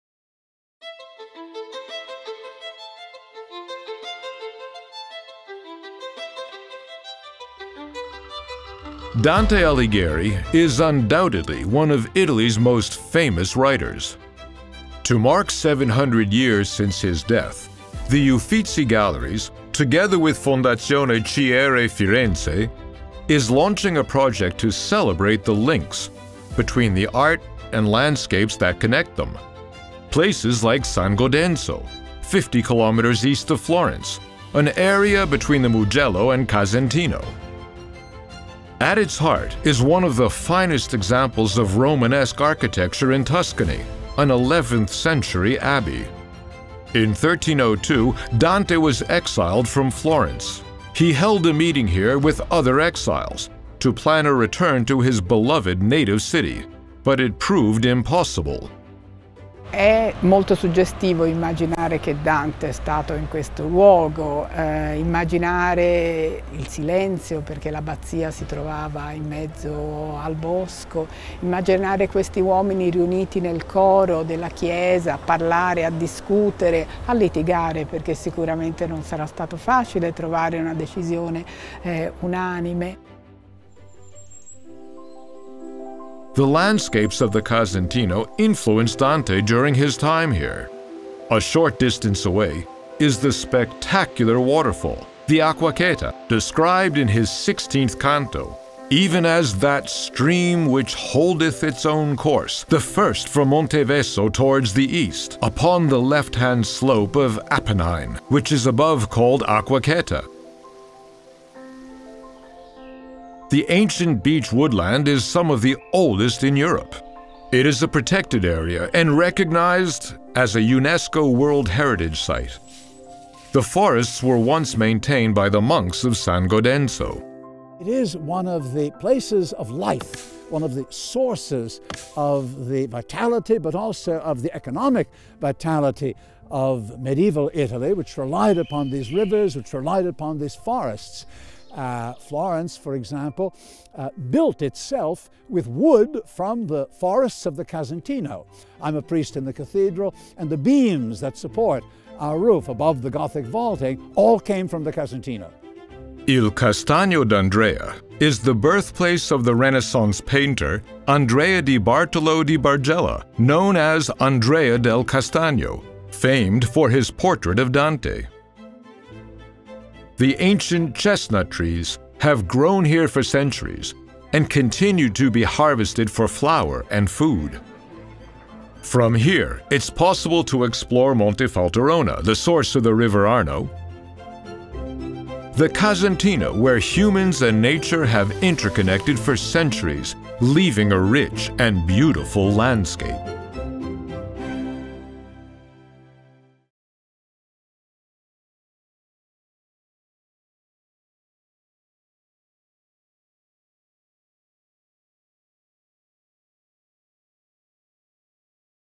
Dante and San Godenzo | Terre degli Uffizi
Nature meets history in the town of San Godenzo, which arose around the eponymous abbey sitting on the border between Tuscany and Emilia-Romagna. Ancient beech forests with their many-hued foliage bathe in the silence of the centuries. The time-worn stones of the Abbey of San Godenzo bear witness to the labours of the monks who tended the chestnut groves and founded the town. Dante and other Florentine exiles met here to plan their return to Florence.